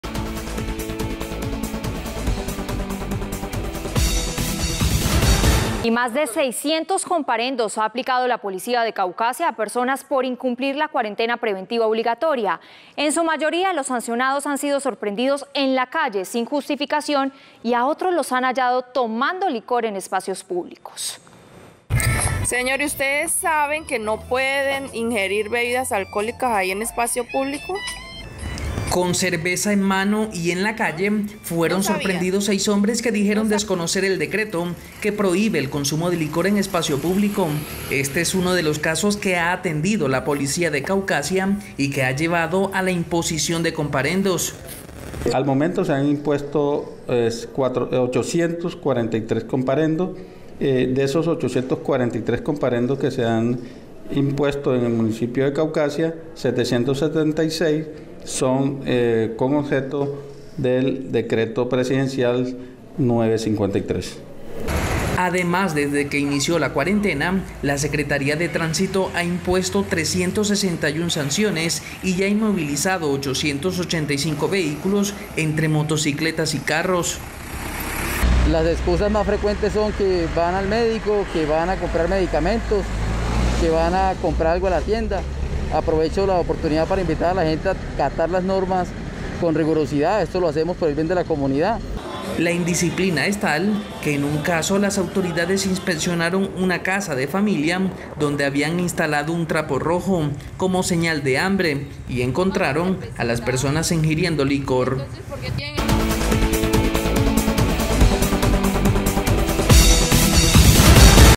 600 comparendos por violar la cuarentena en Caucasia - Teleantioquia Noticias
Más de 600 comparendos ha aplicado la Policía de Caucasia a personas por incumplir la cuarentena preventiva obligatoria. En su mayoría, los sancionados han sido sorprendidos en la calle sin justificación y otros tomando licor en espacios públicos.